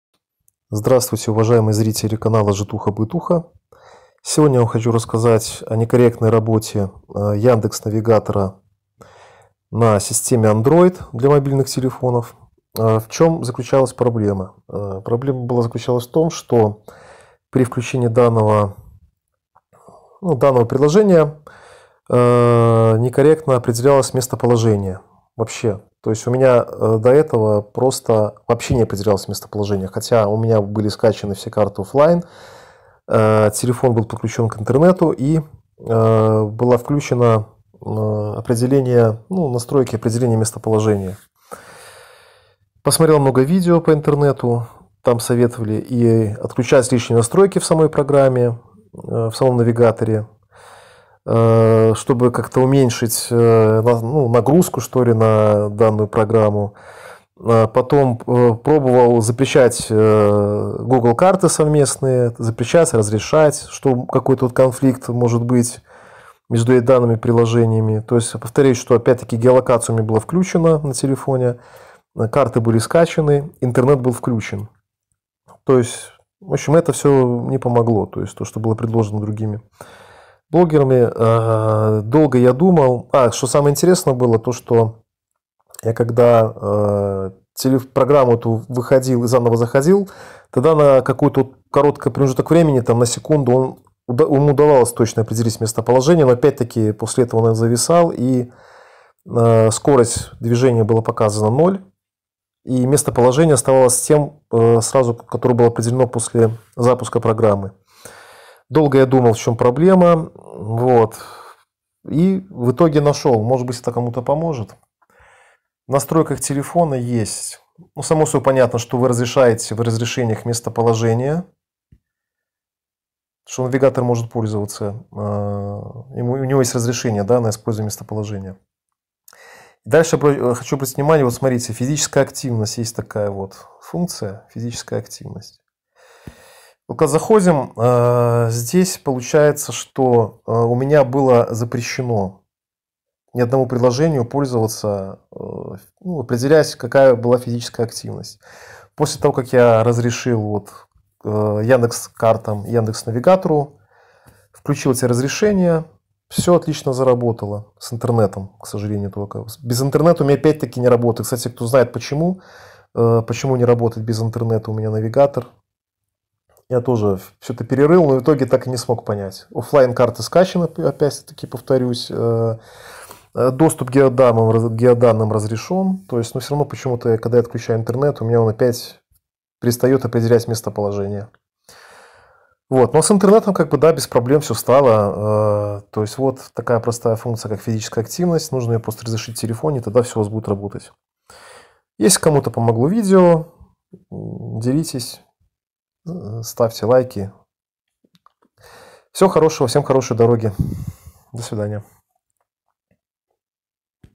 Мобильный навигатор не определяет местоположение / Глючит навигатор Яндекс
В этом видео поделюсь своим опытом настройки Яндекс Навигатора, который неправильно определял мое местоположение. Прошерстив кучу страниц интернета и пересмотрев много видео, я так и не смог найти подсказку этой причины. Причина определения неверного местоположения обнаружилась случайно, когда стал экспериментировать с настойками телефона. После установки определенных разрешений Яндекс навигатор заработал корректным образом.